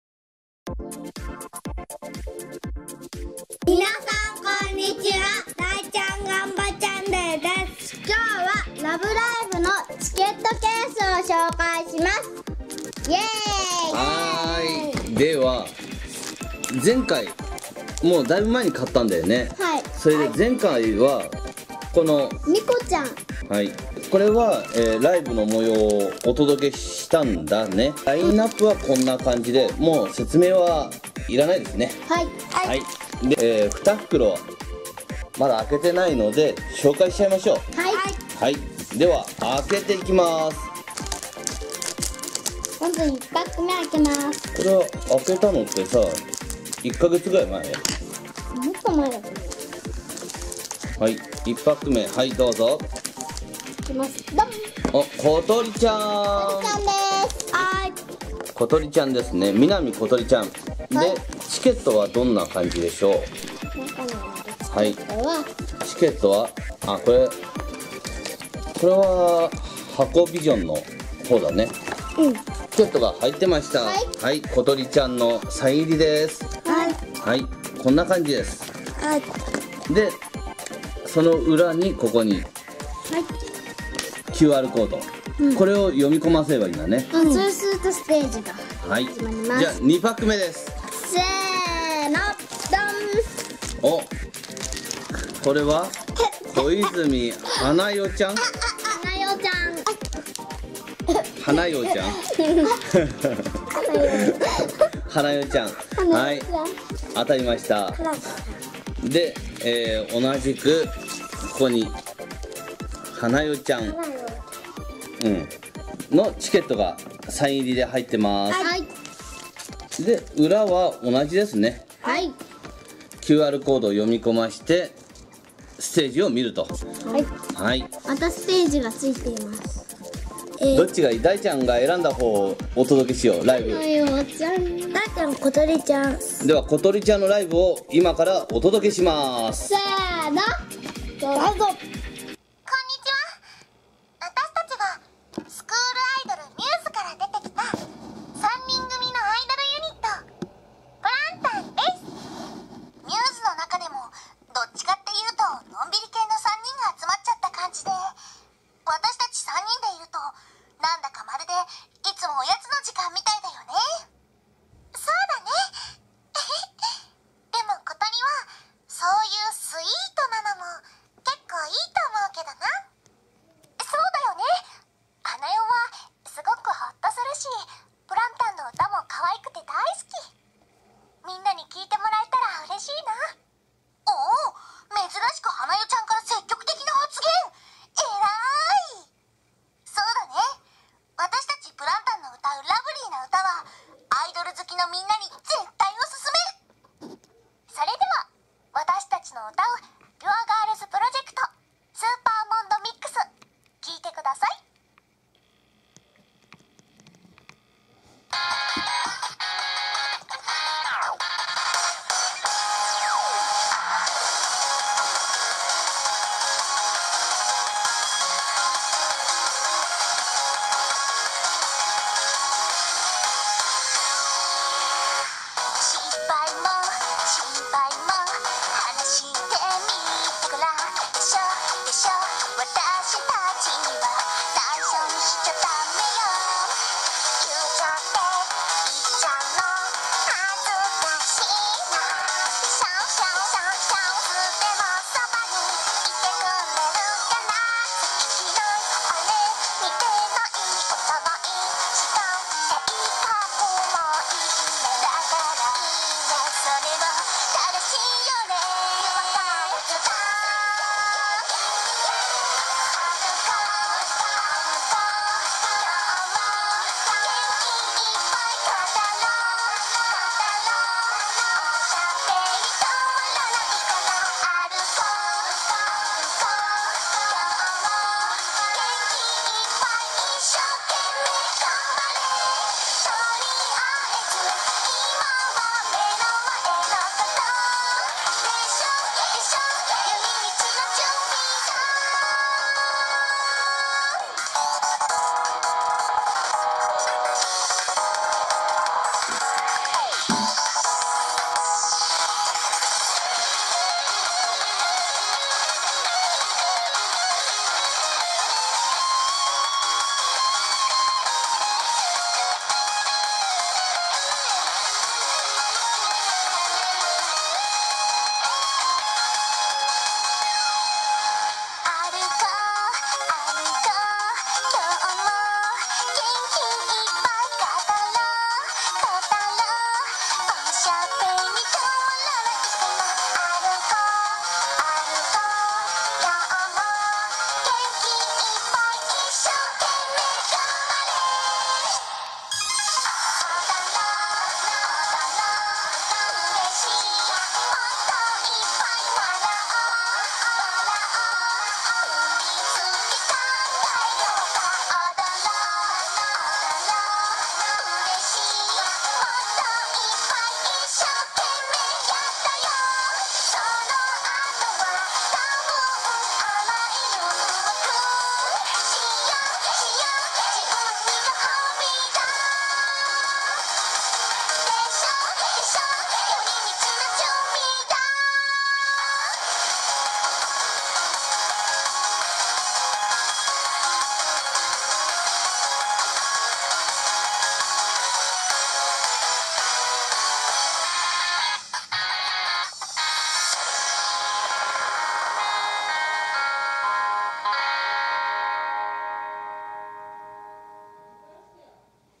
ラヴライブチケットケース２パック紹介【特典ライブ映像あります♬】
説明・ラヴライブチケットケース２パック紹介【特典ライブ映像あります】欲しいキャラが手に入りました！